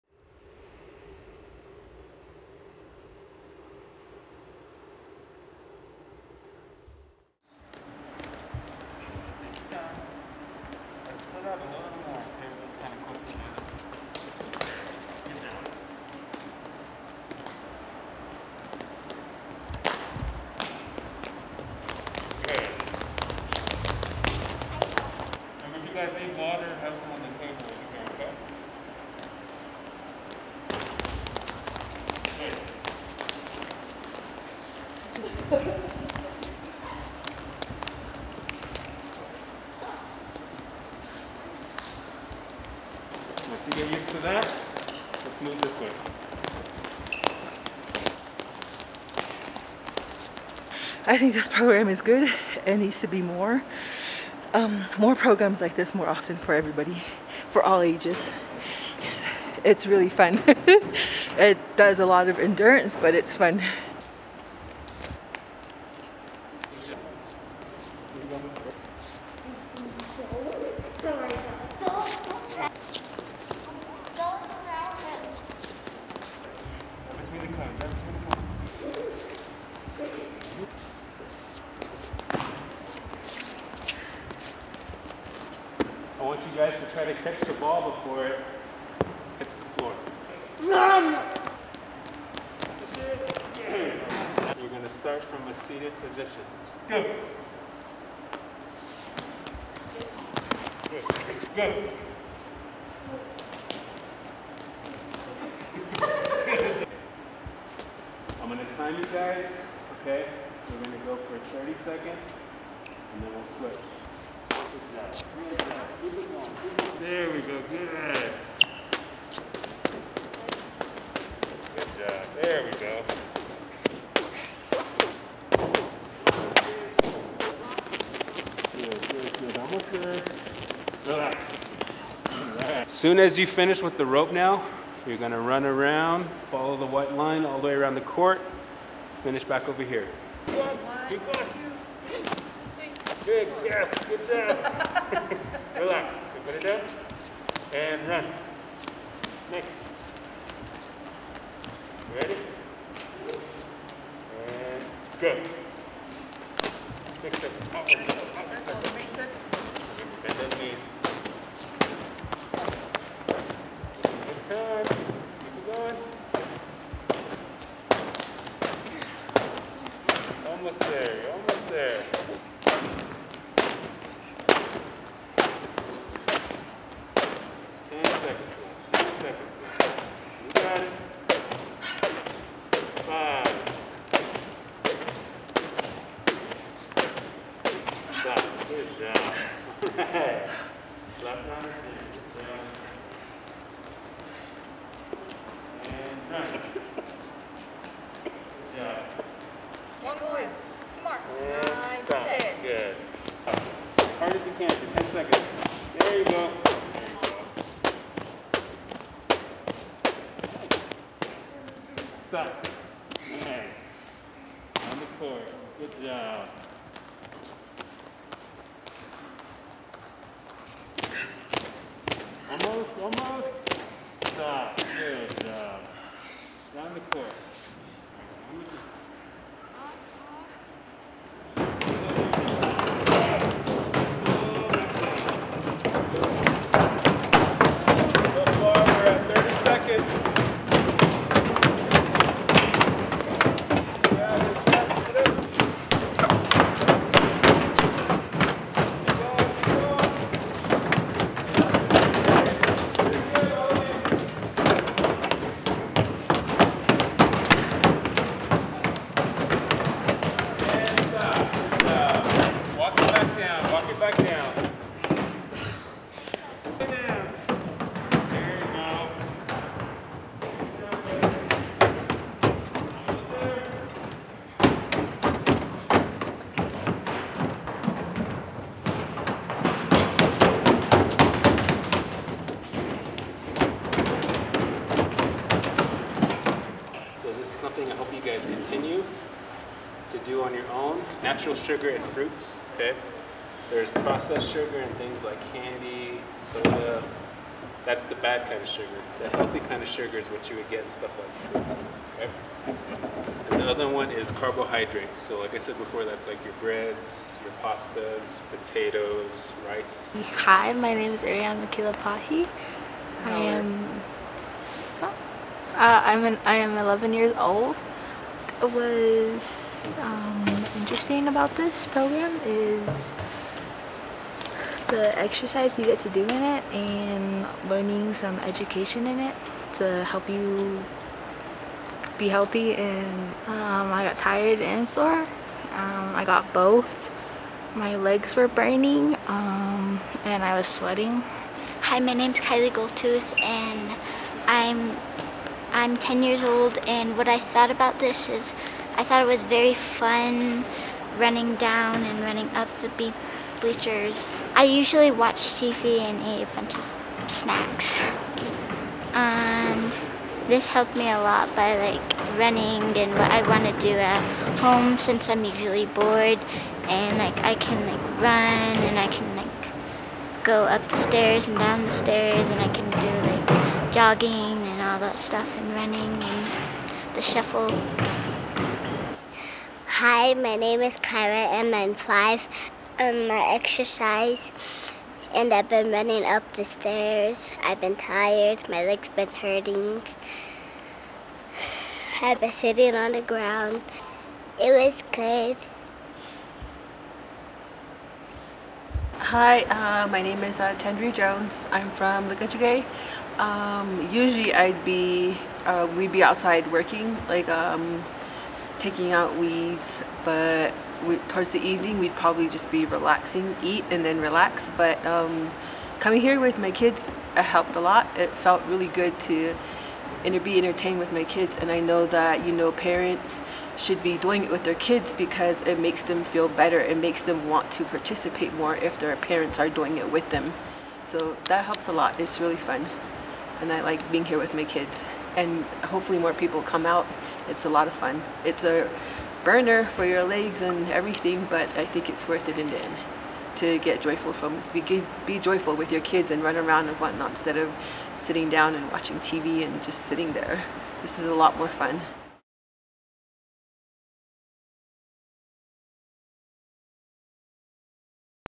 NNSDP 2023 Summer Youth Fitness Camp
The Navajo Nation Special Diabetes Program is FEDERALYY funded to provide diabetes education and prevention through physical activity and nutrition education on the Navajo Nation. Herein, is a fitness camp held at the Lukachukai Community School in Arizona. Diabetes is preventable.